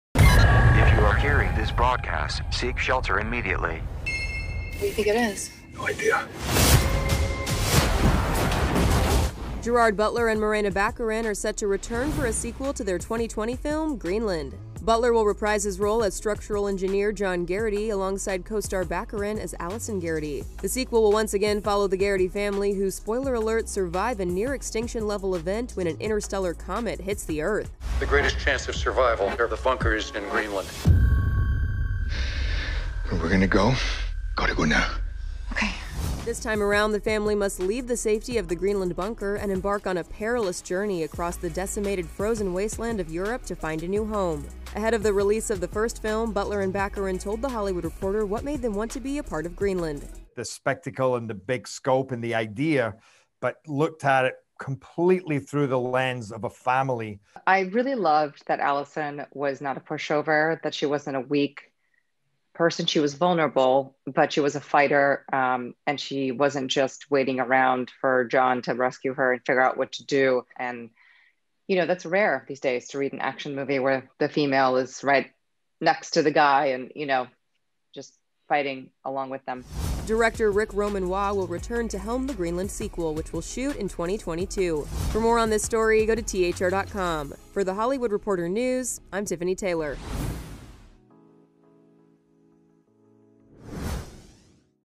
Gerard Butler, Morena Baccarin Starring in ‘Greenland 2’ – THR News
Butler will reprise his role as structural engineer John Garrity, alongside co-star Morena Baccarin as Allison Garrity. Director Ric Roman Waugh will return to helm the sequel.